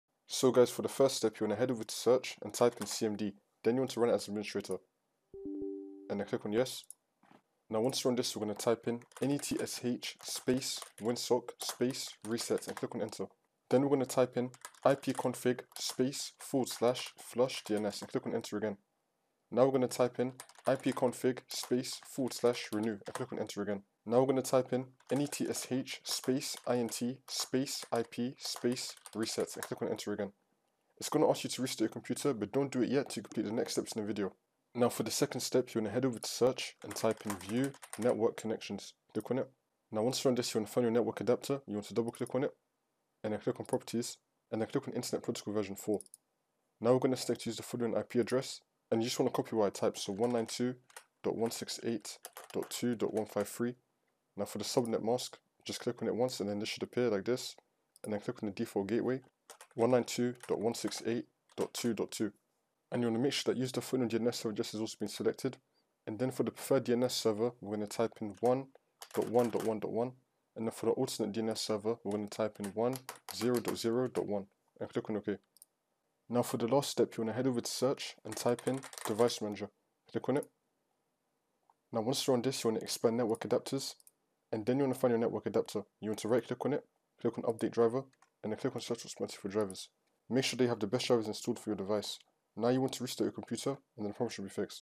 How To Fix Unidentified Network No Internet Access in Windows 11
How to Fix Unidentified Network No Internet Access Windows 11

ipconfig /flushdns
ipconfig /renew

Learn how to resolve the issue of "Unidentified Network No Internet Access" on your Windows 11 PC. This comprehensive guide provides step-by-step solutions to troubleshoot and fix connectivity problems, ensuring you regain internet access on your device. Follow our troubleshooting steps to quickly resolve the unidentified network issue.